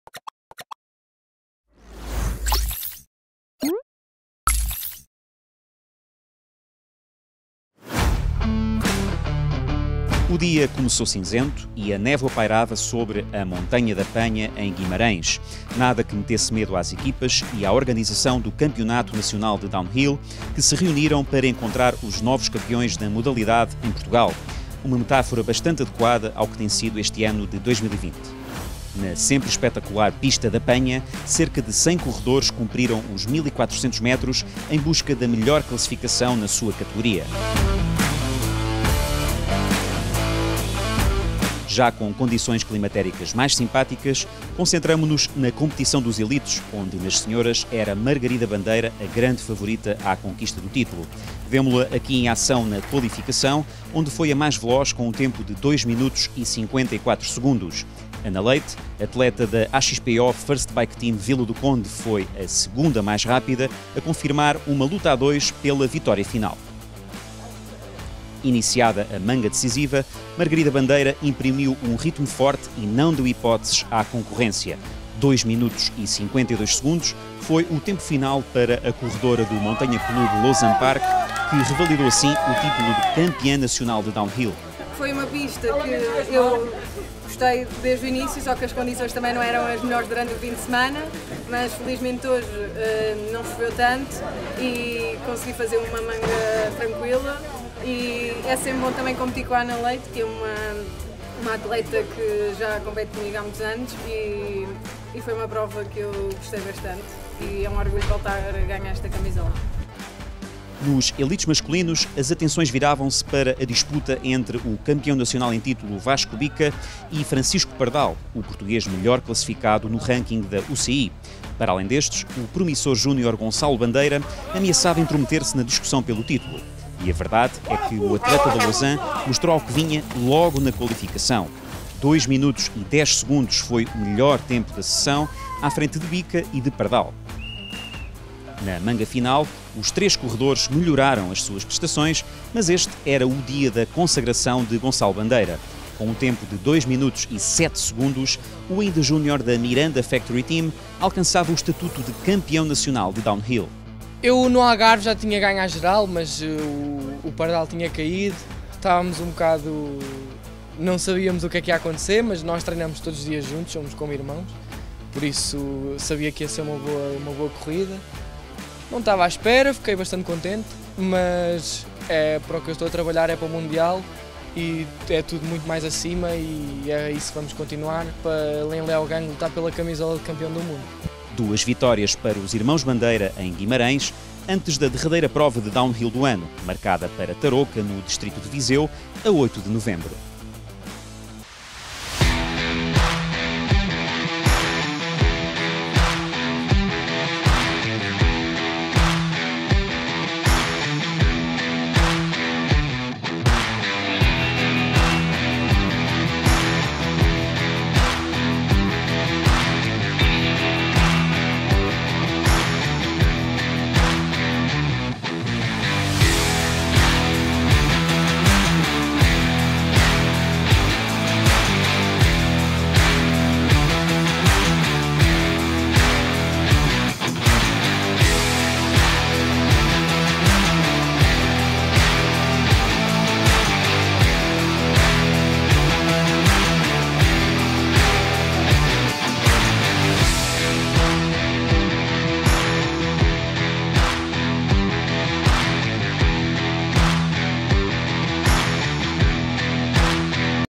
Campeonato Nacional de DHI de 2020 - Penha (Guimarães)
Resumo do Campeonato Nacional de DHI de 2020, disputado na Penha, em Guimarães.
Em breve, resumo alargado na RTP2